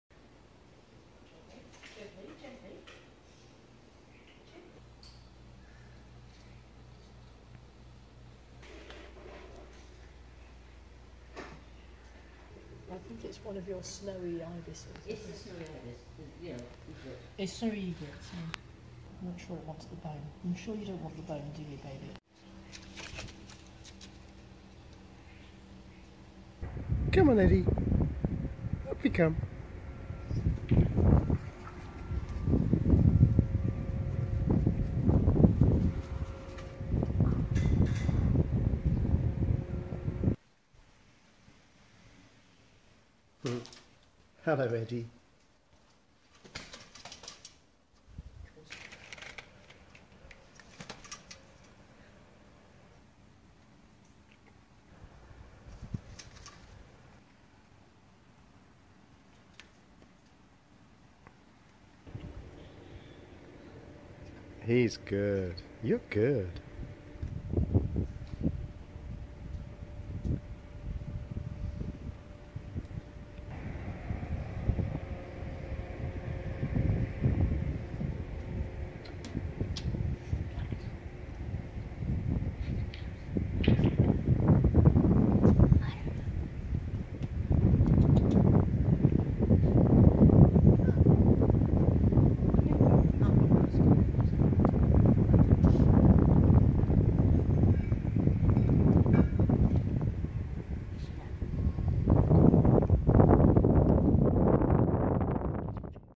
snowy egret
An opportunistic snowy egret in Fort Meyers Beach, Florida